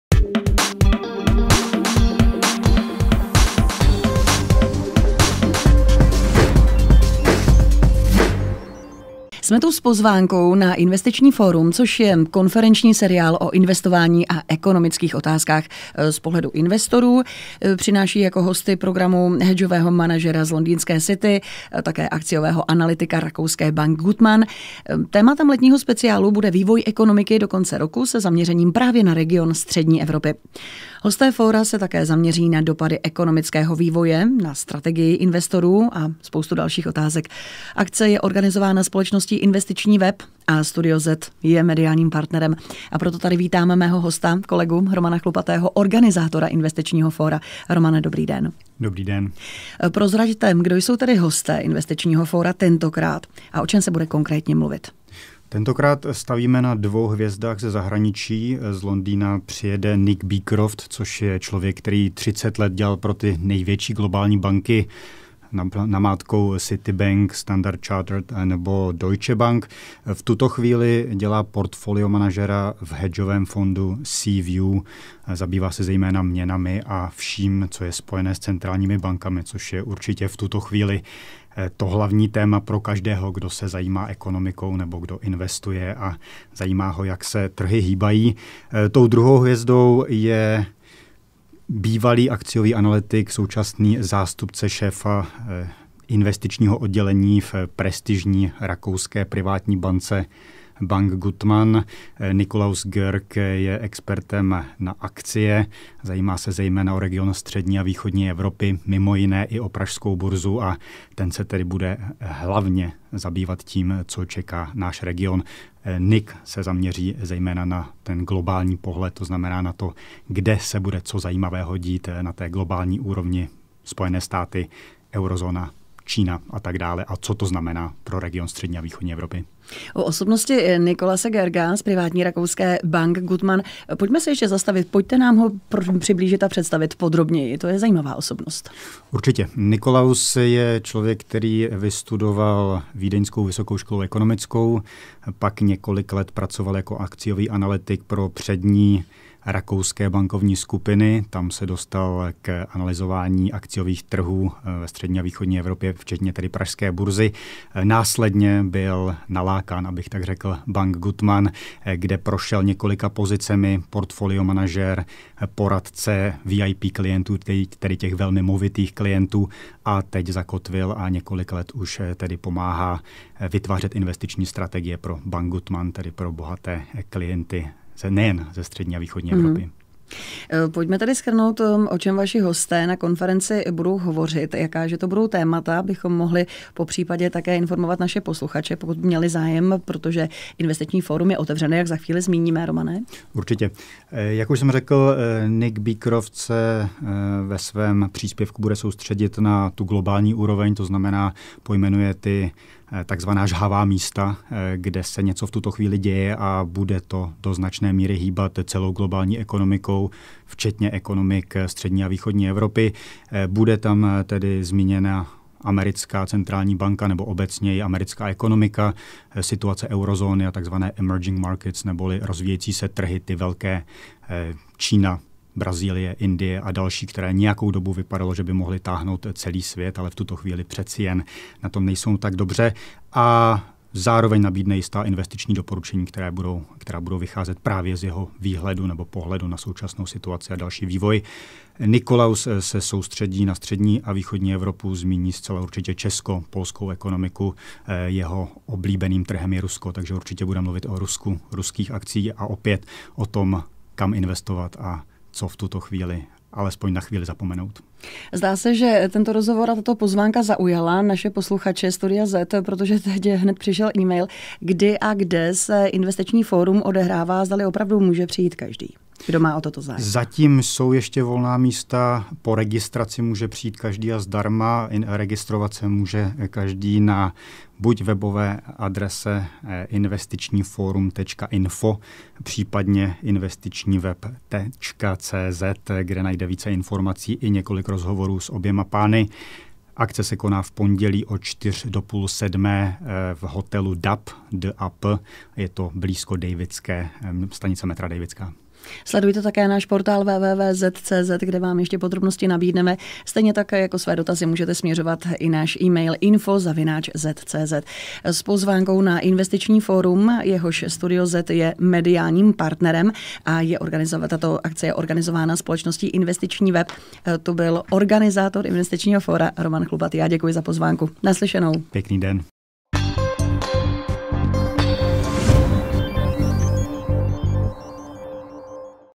Investiční fórum Speciál: Co čeká svět investic i náš region konkrétně do konce roku?
Investiční web pro vás připravil Investiční fórum Speciál, na němž vystoupí Nick Beecroft, uznávaný hedgeový manažer z londýnské City, a Nikolaus Görg, akciový analytik z prestižní privátní rakouské Bank Gutmann. Oba nabídnou svá očekávání ohledně zbytku roku a odhalí strategie, jimiž se oni a jejich klienti budou řídit. V rámci svých prezentací mají vyhrazen čas také na diskuzi s diváky. Registrovat se můžete na adrese